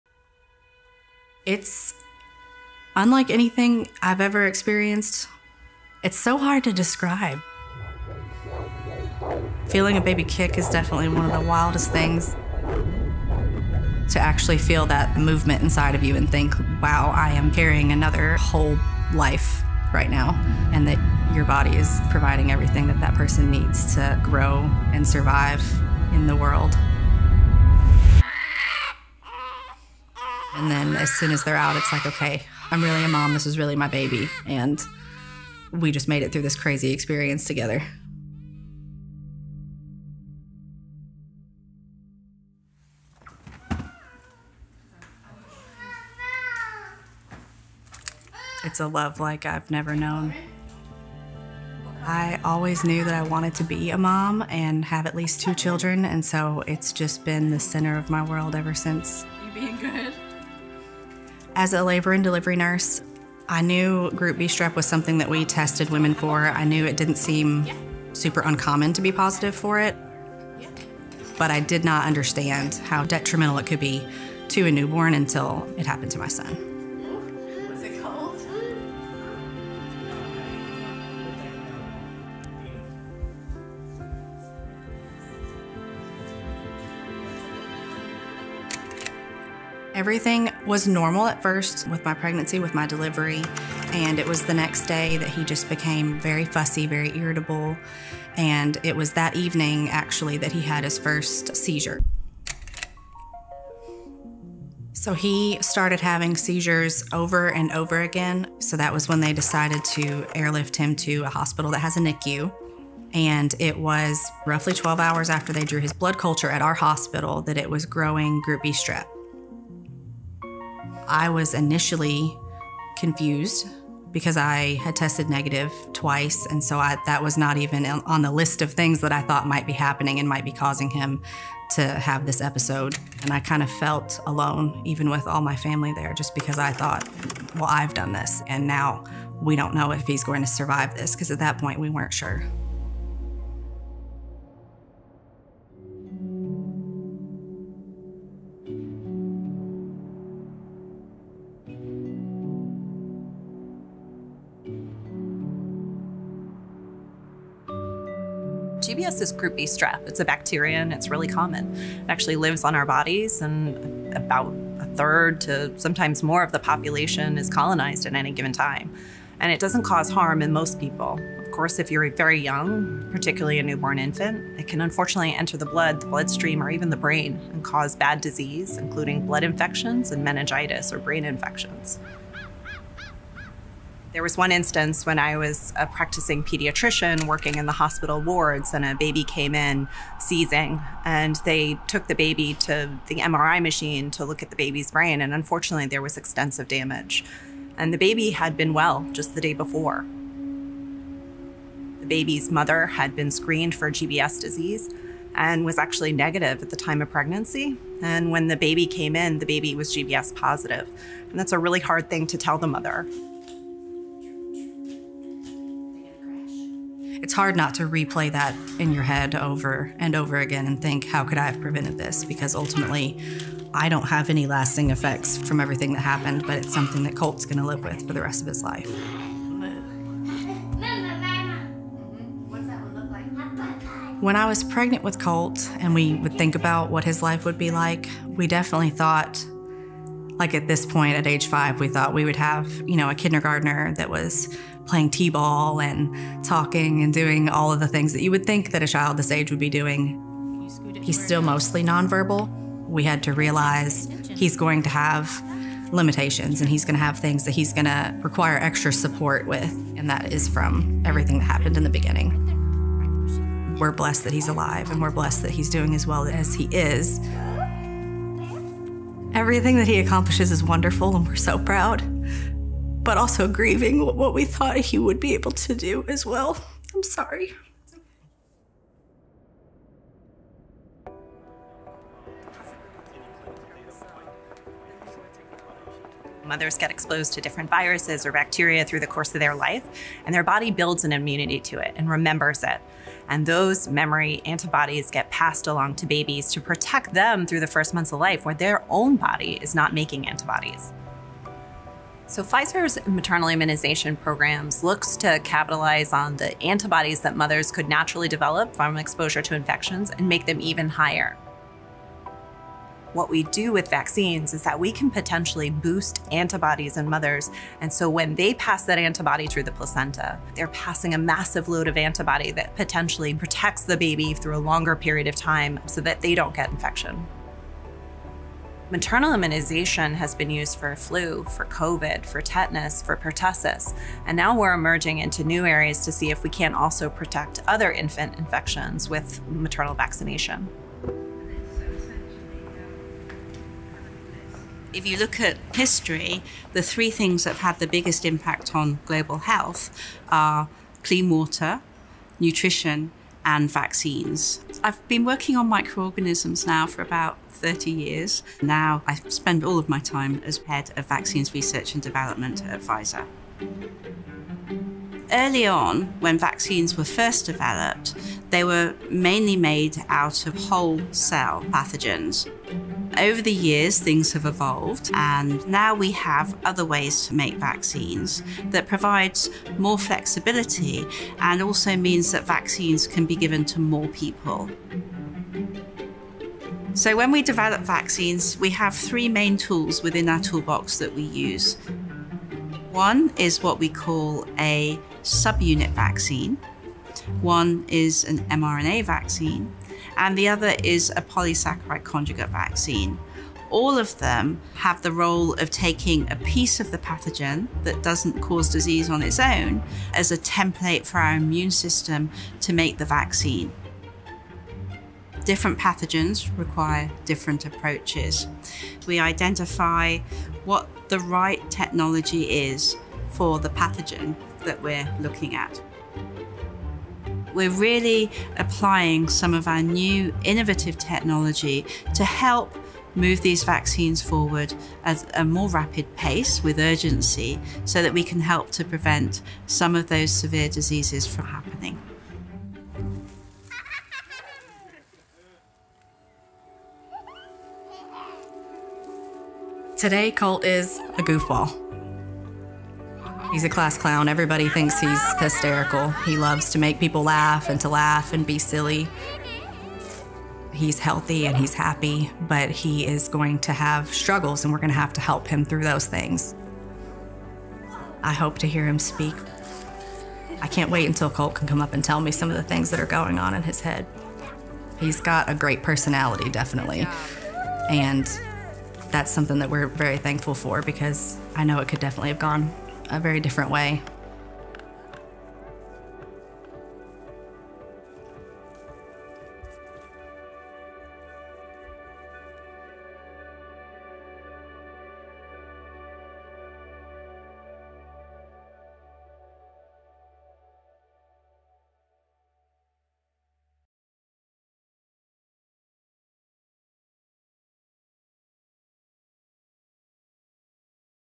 How Immunization Can Help Protect New Lives
Viruses and bacteria that can be relatively harmless to the average adult can be life-threatening to young infants whose immune systems are often not strong enough to protect against infections. In this HealthierTogether video, learn more about the need for additional tools, such as maternal immunization, to continue to fight infectious diseases as well as the innovative vaccine technologies we believe can help protect more people against such diseases.